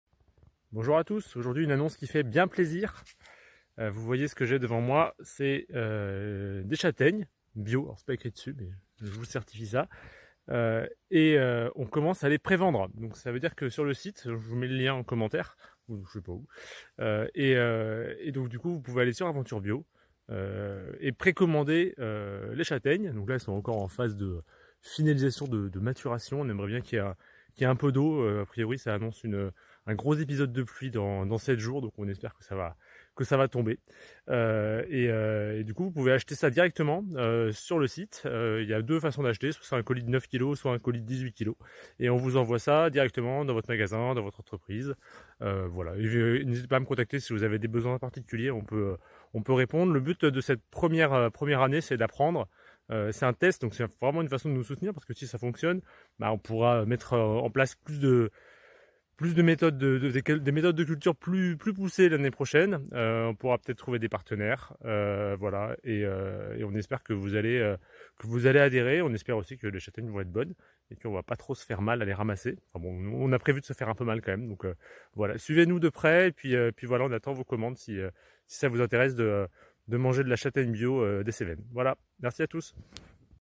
Pré-commandez nos châtaignes bio des Cévennes, expédition direct de notre châtaigneraie !
Nos châtaignes certifiées bio, des Cévennes, sont en train de mûrir, pour commander en expédition direct de notre châtaigneraie cliquez ci dessous